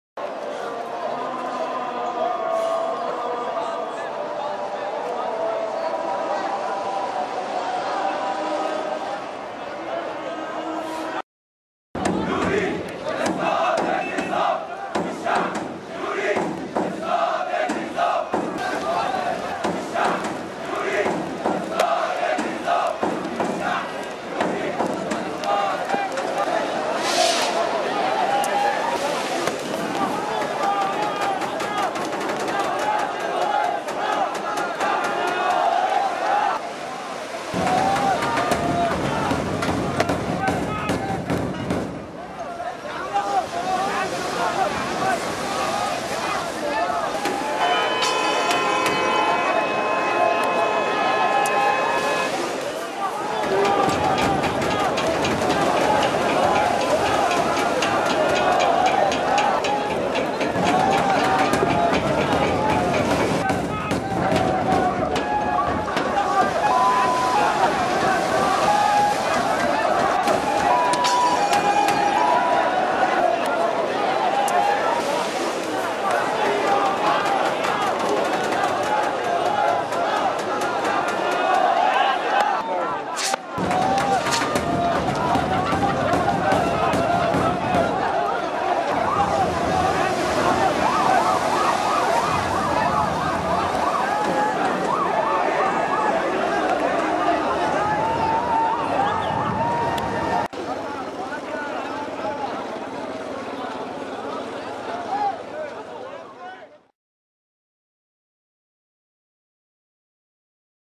Protests outside the Alexandria Criminal Court
Clashes broke outside Alexandria Criminal Court in Egypt's second city, on 20th January 2013 in protest against certain police and security personnel, who they accuse of killing demonstrators during the 2011 revolution. The Corniche, which runs along the seafront, saw protesters hurling sticks and rocks, cars set alight and fireworks set off. The police retaliated with tear gas and rubber bullets.